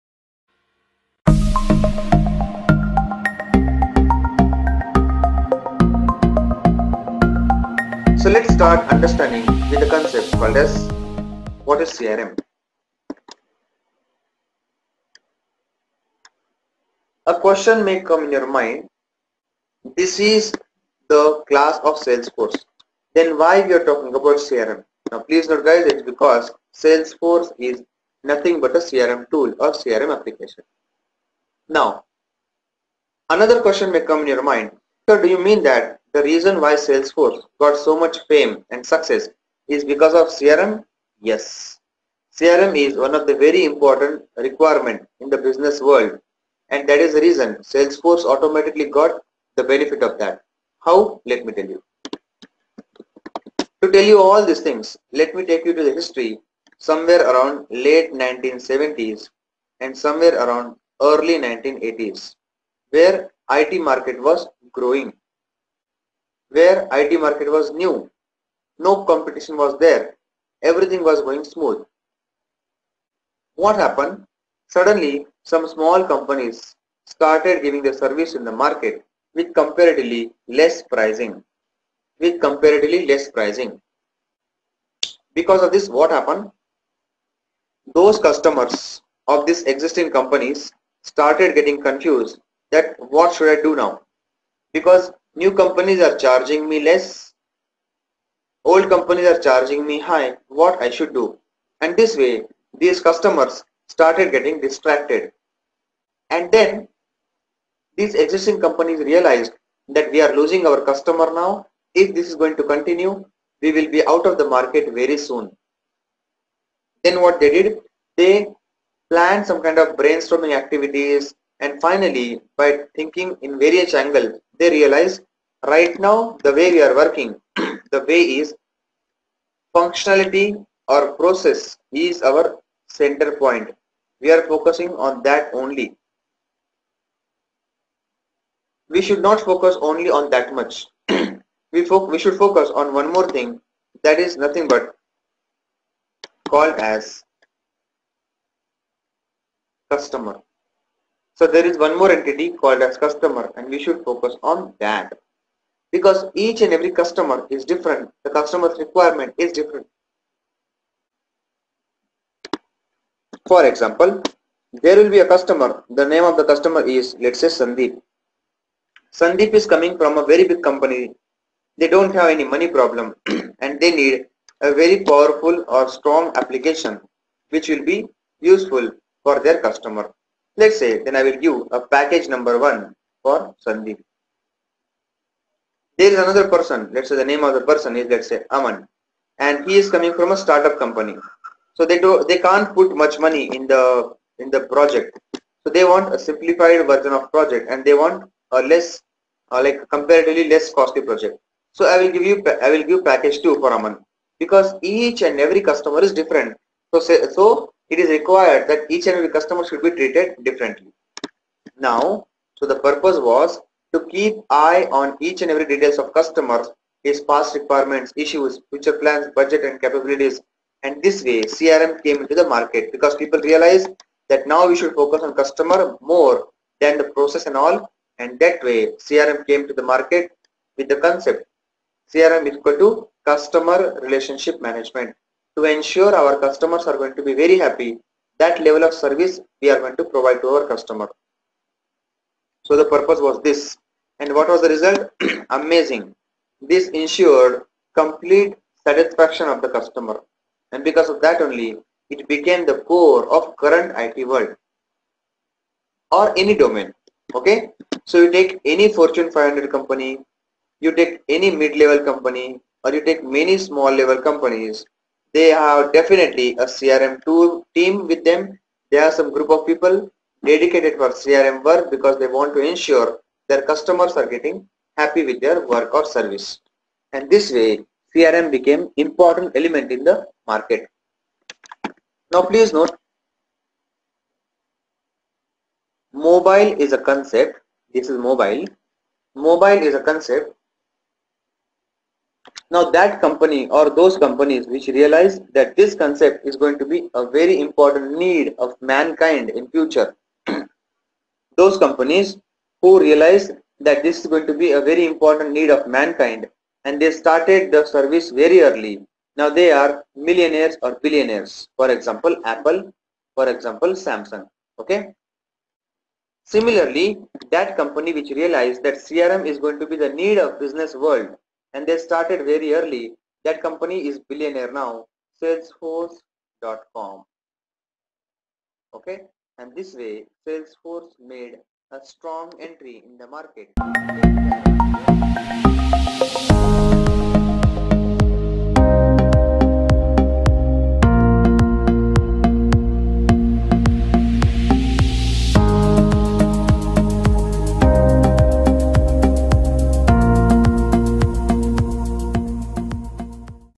What is CRM (Customer Relationship Management) | Salesforce Admin Tutorial
What is CRM (Customer Relationship Management) | Salesforce Certification Videos
CRM stands for CUSTOMER RELATIONSHIP MANAGEMENT.CRM is a technology for managing all the company's relationships and interactions with customers and potential customers and stores customer contact information.It actively tracks and manages the customer information.It connects the entire team with the simple goal to improve business relationships. It also delivers the instant insights and recommendations.

What is CRM?

Core of IT Industry
Purpose
 Keeps an eye on each and every details of customer
 Past requirements, issues, future plans
Result
 Ensures customer’s complete satisfaction
Salesforce is a CRM tool